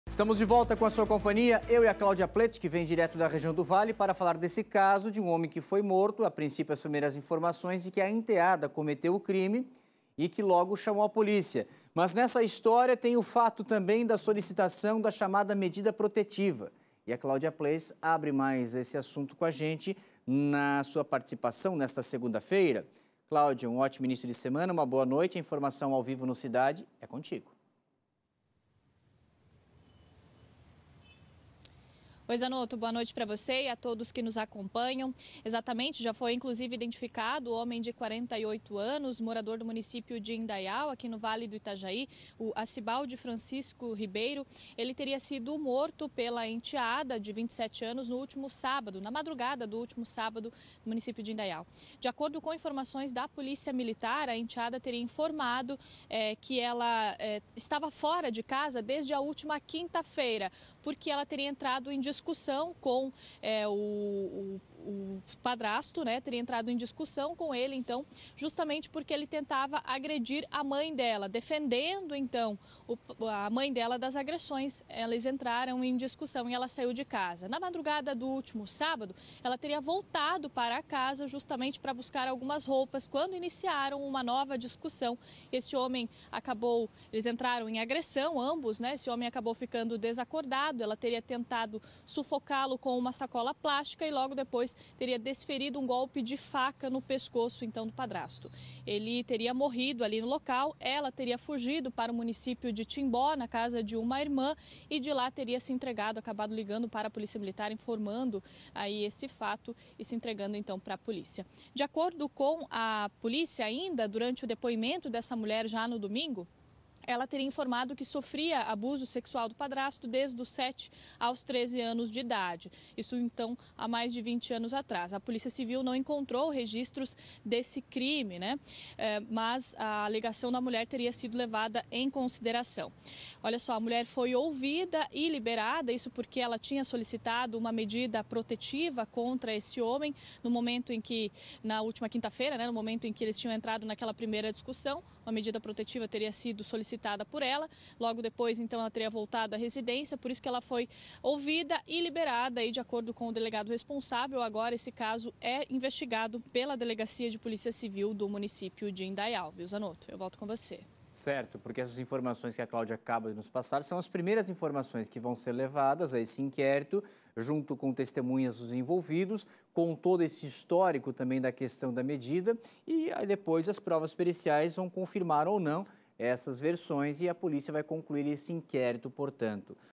Matou o padrasto e se entregou: suspeita pediu medida protetiva dois dias antes
O crime foi registrado na cidade de Indaial. A enteada disse que além de já ter sido abusada quando era criança, também tinha medida protetiva contra o homem. Confira.